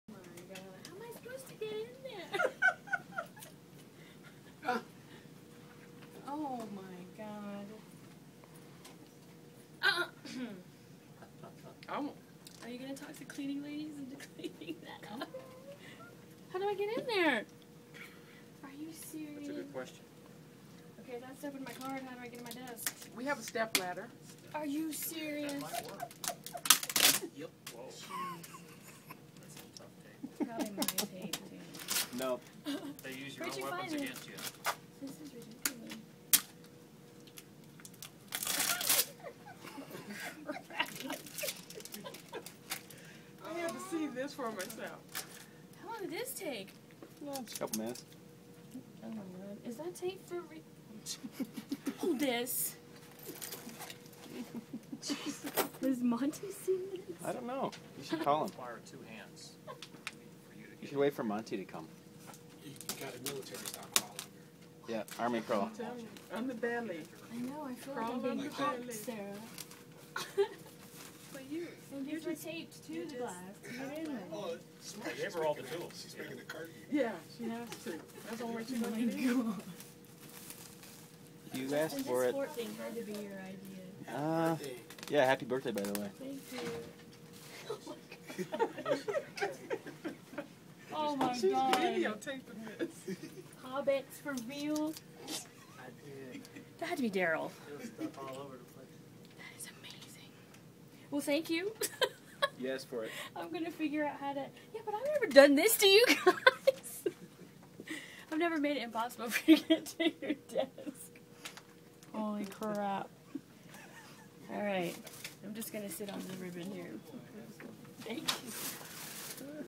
Roni's birthday cubicle
Cubicle decorated for Roni's birthday.